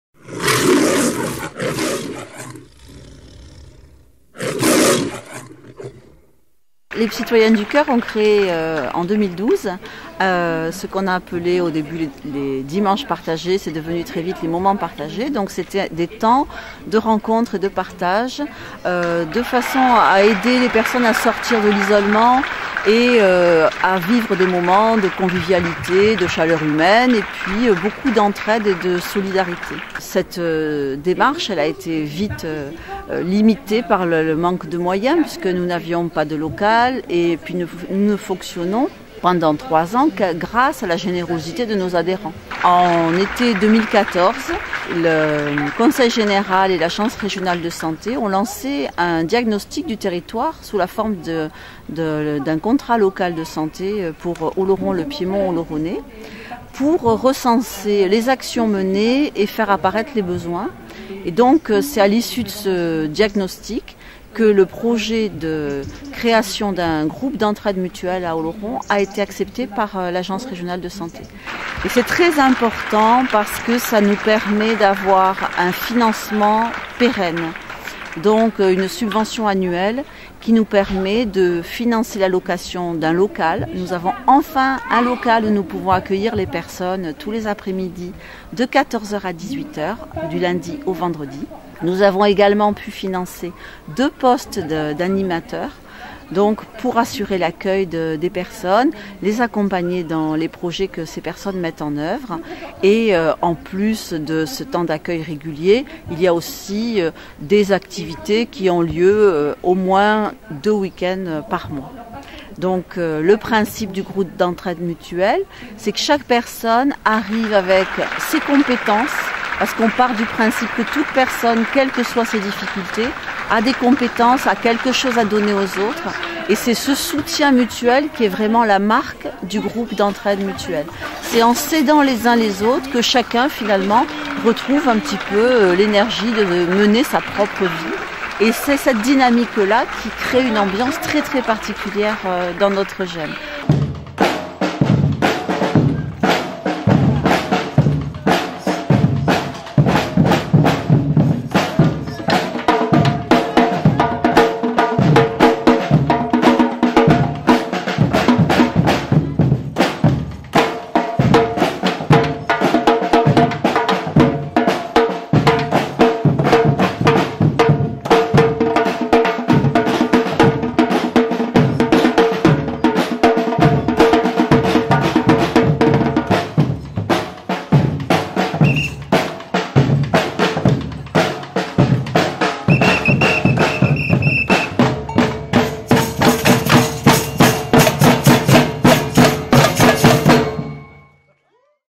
Psytoyens du coeur inauguration (GEM)
Ce nouveau dispositif porte le nom de groupe d'entraide mutuelle (GEM). Son objectif est l'amélioration des conditions de vie de ces personnes.